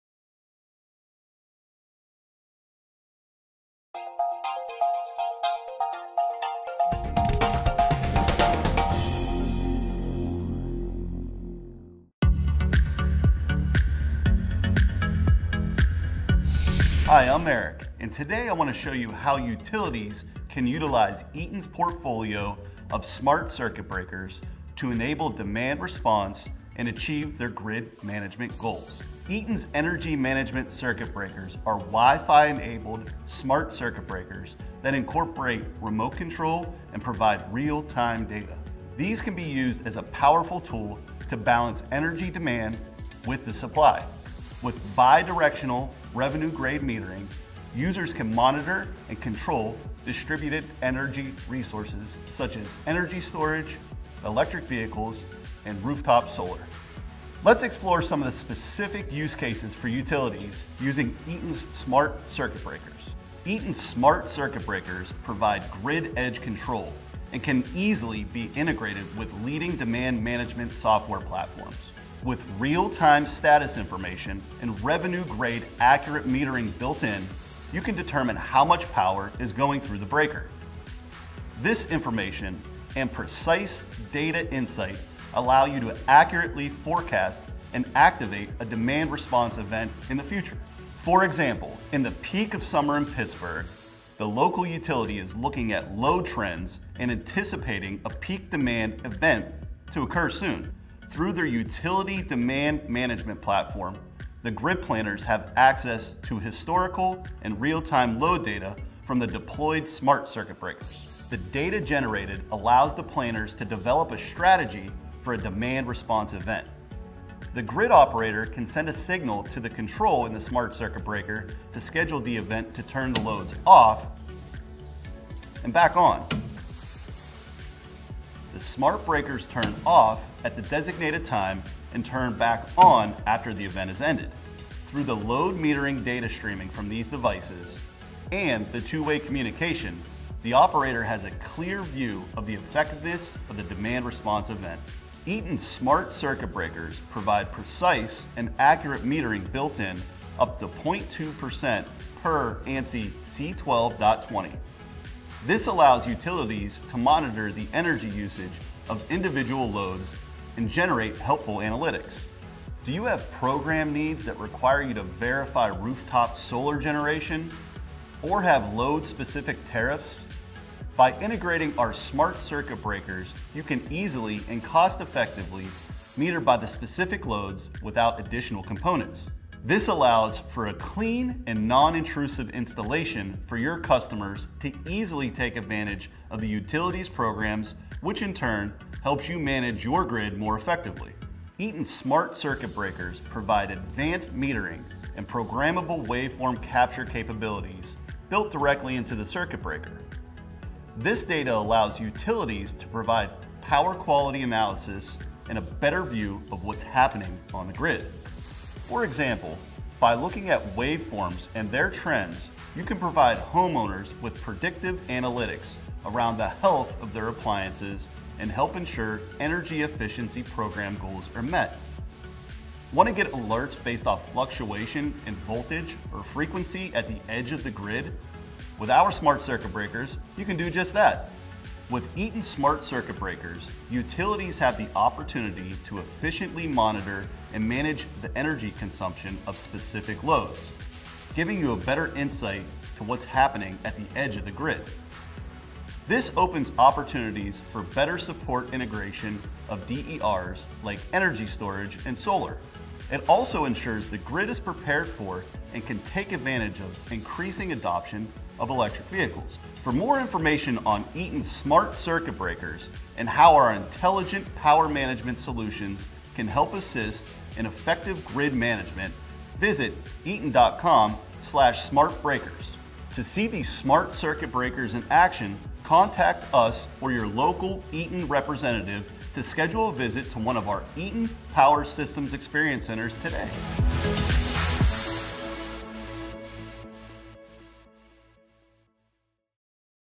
Eaton smart circuit breakers and utility demand response programs
Eaton smart circuit breakers offer homeowners and utility companies opportunities to manage loads for demand response programs including EV charging, water heaters, HVAC and more.  Monitoring and control all in a WIFI enabled residential breaker.  Managing customer load demands remotely with these smart breakers allows utilities the ability to offer more control and incentives to customers that want to participate in the program.  smartgrid, load shedding, demand response